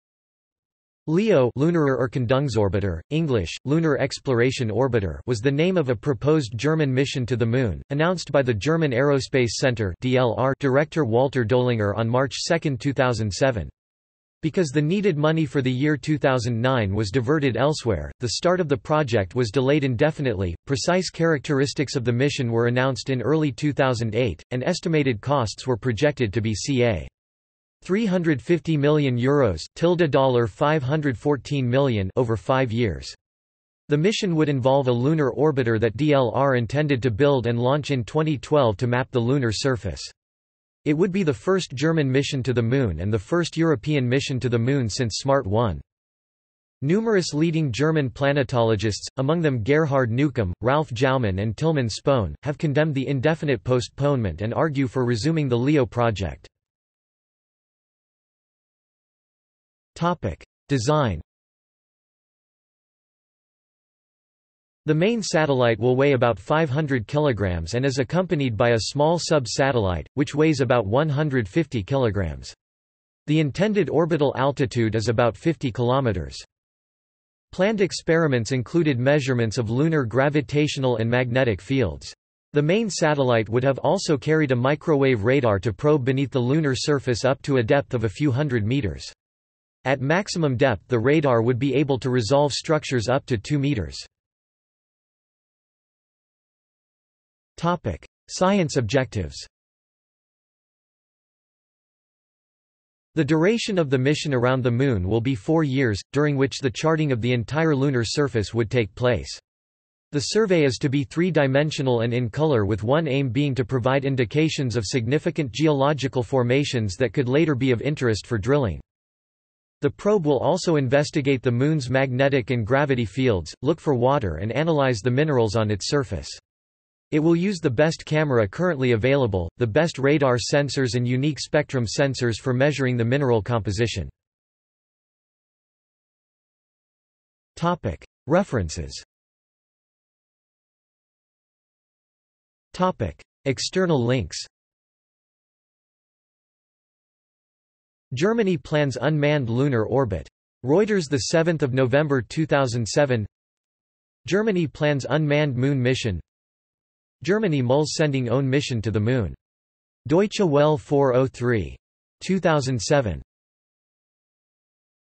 LEO (spacecraft) | Wikipedia audio article
This is an audio version of the Wikipedia Article:
LEO (spacecraft)

- Socrates

SUMMARY
LEO (Lunarer Erkundungsorbiter; English: Lunar Exploration Orbiter) was the name of a proposed German mission to the Moon, announced by the German Aerospace Center (DLR) Director Walter Doellinger on March 2, 2007. Because the needed money for the year 2009 was diverted elsewhere, the start of the project was delayed indefinitely.Precise characteristics of the mission were announced in early 2008, and estimated costs were projected to be ca. €350 million (~$514 million) over five years. The mission would involve a lunar orbiter that DLR intended to build and launch in 2012 to map the lunar surface. It would be the first German mission to the Moon and the first European mission to the Moon since SMART-1.
Numerous leading German planetologists, among them Gerhard Neukum, Ralf Jaumann and Tilman Spohn, have condemned the indefinite postponement and argue for resuming the LEO-project.